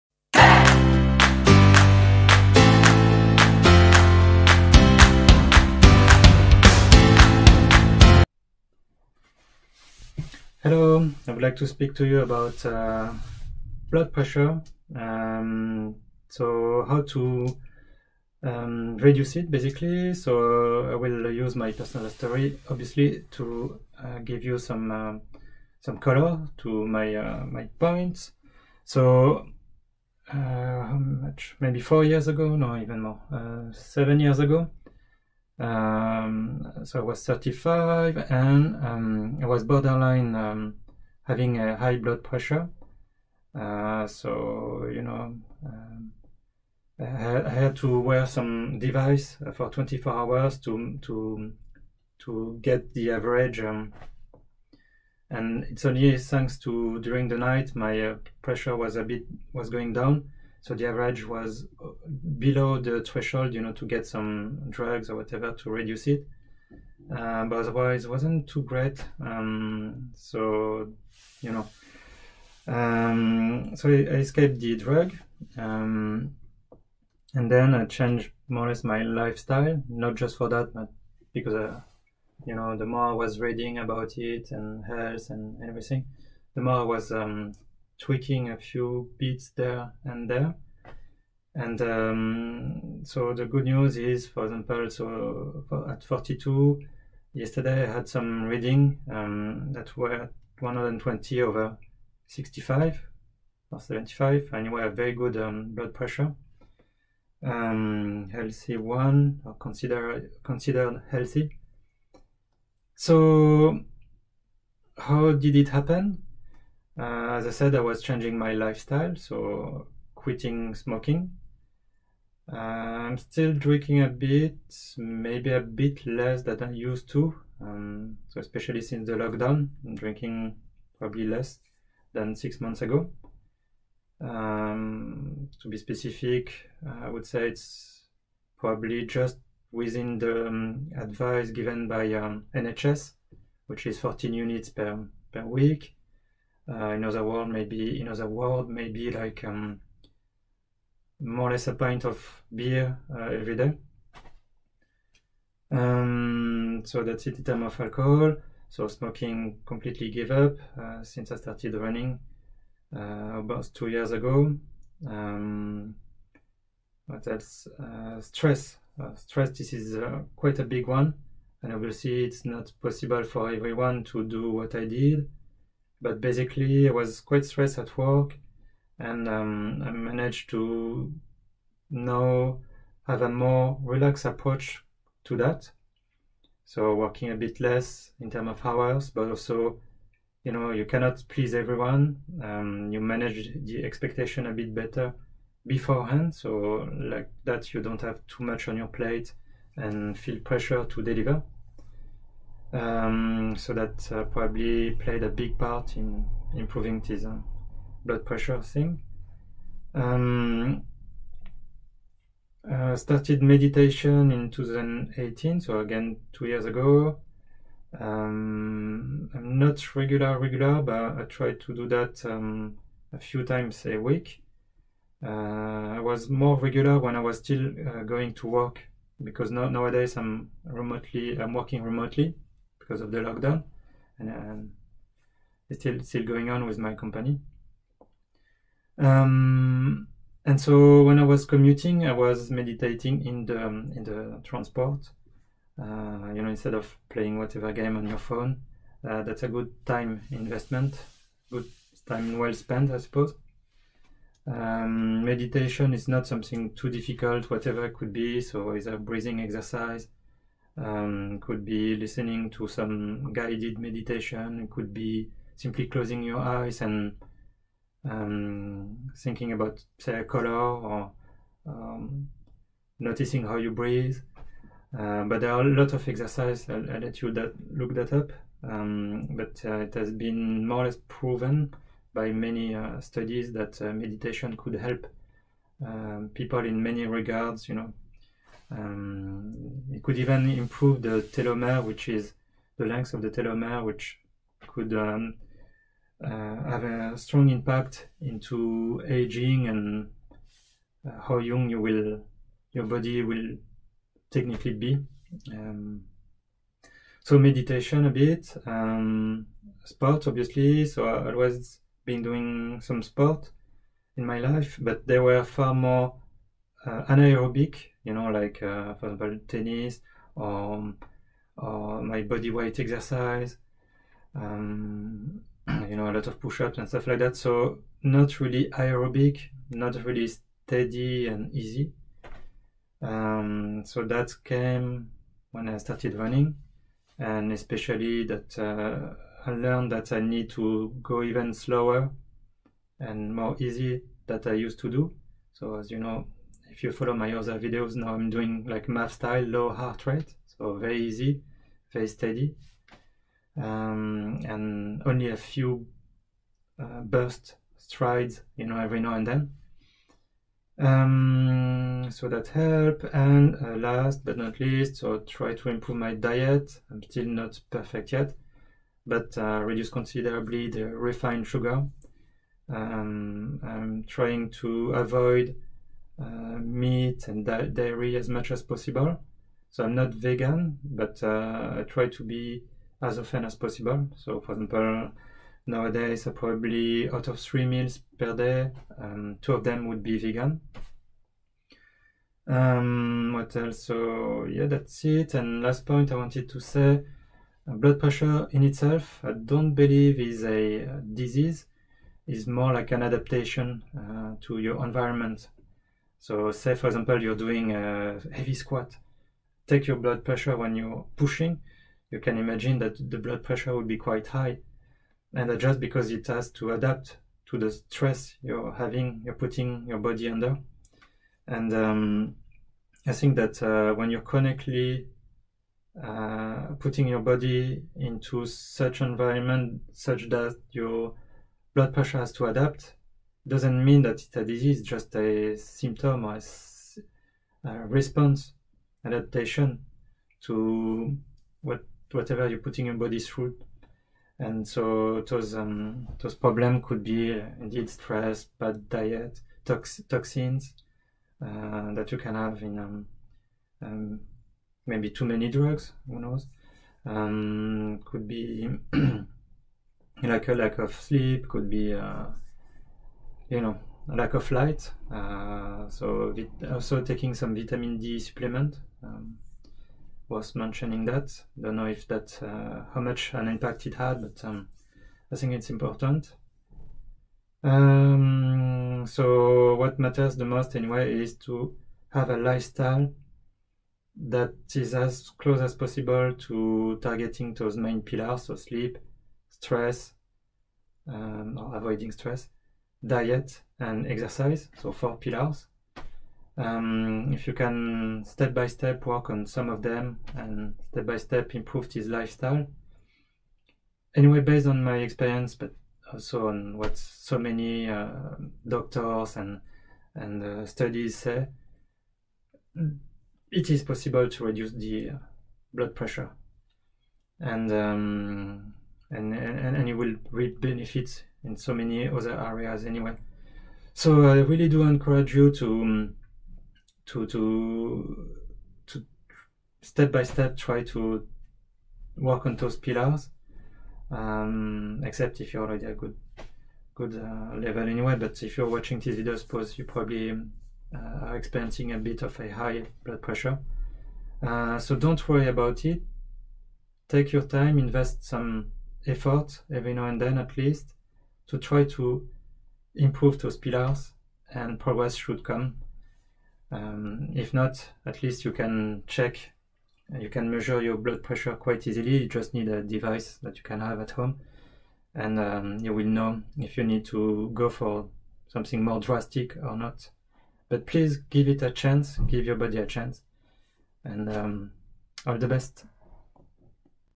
How to reduce Blood Pressure
It takes time to reduce blood pressure without drugs.

In essence it boils down to having a healthy lifestyle, which is based on 4 pillars:
1) Good diet (low refined carbs, process food, alcohol, lots of vegs)
2) Exercise (aerobic preferably, even once a week or simply walking is better than nothing)
3) Good sleep (6-8 hours fairly continuous)
4) Low stress (avoidance, meditation, sense of community)